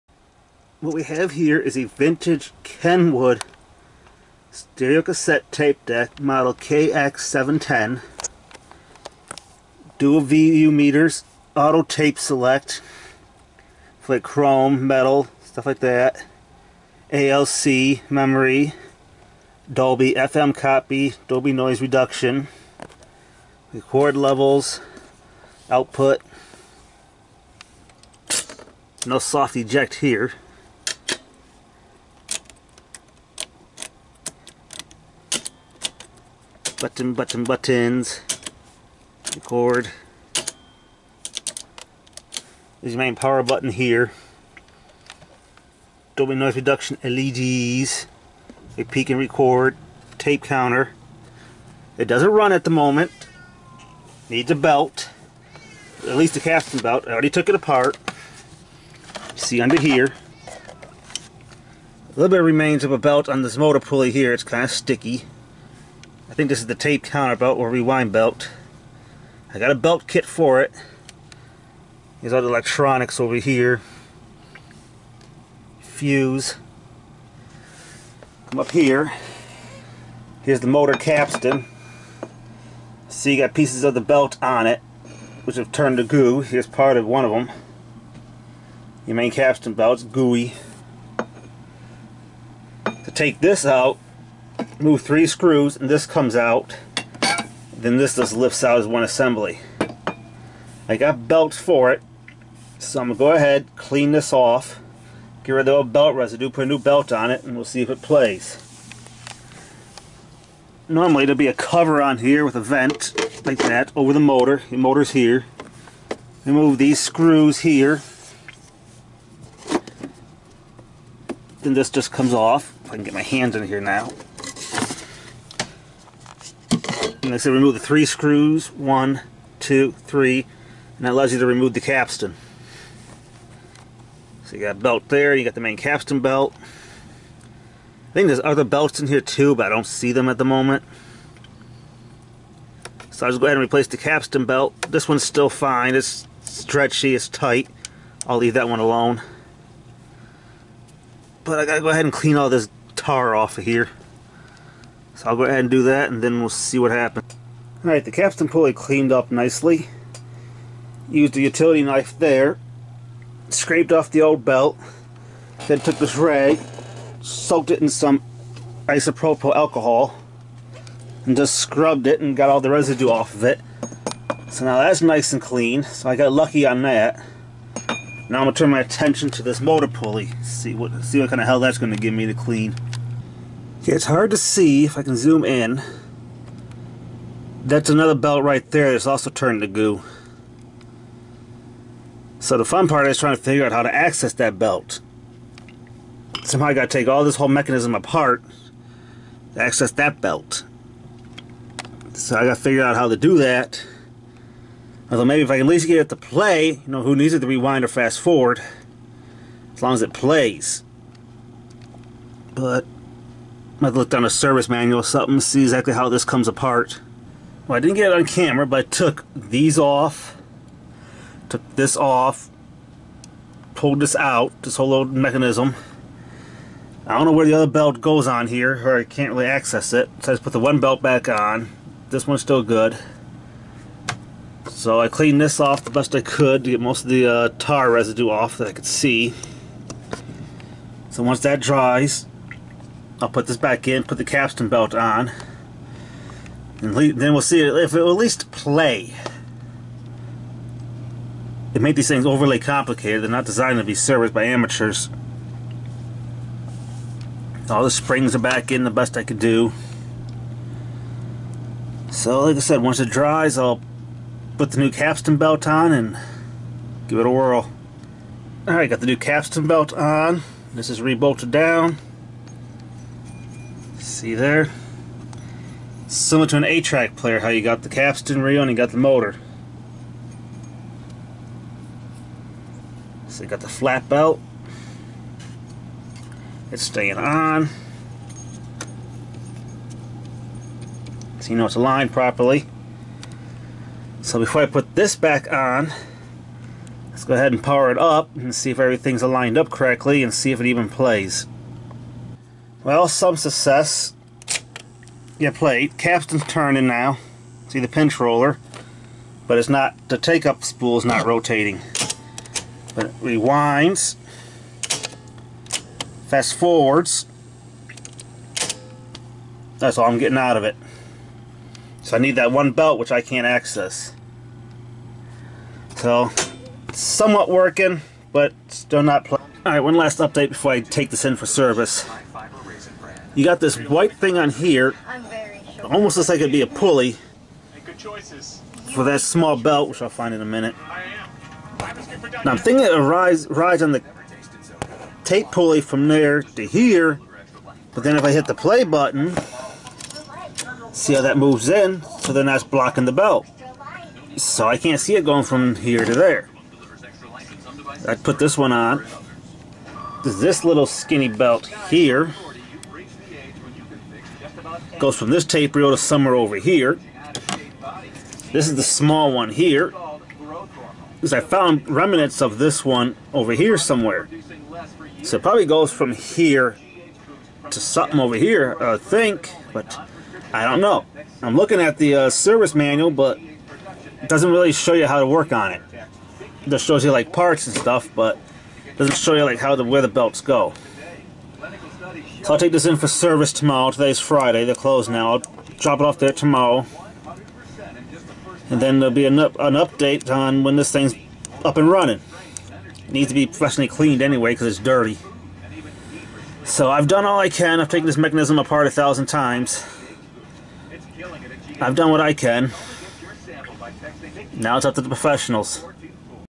Vintage Kenwood KX-710 Cassette Deck Repair Part 1
Found this on ebay. Ad stated everything worked fine it just needed a belt. I ordered a belt kit for it as this machine takes 4 belts. When I opened it up I found the capstan belt had tuned to goo.

I was able to scrape the old belt off and use isopropyl alcohol to clean the belt residue off. Found another belt underneath the deck that had also turned to goo but I cannot figure out where it goes. I checked the service manual I found online and it does not show where this belt goes.

The machine now rewinds and fast forwards and the pinch roller works but the takeup spindle does not turn in play mode. If I could figure out were this belt goes I could get this working.

It looks like it goes on a pulley that's on the capstan shaft but when you press play a bar slides up into the way of the belt so that can't be it. I'm going to take this unit in for repair and we will see what they say. Part 2 will be a update on this unit. Hopefully professionally repaired and working.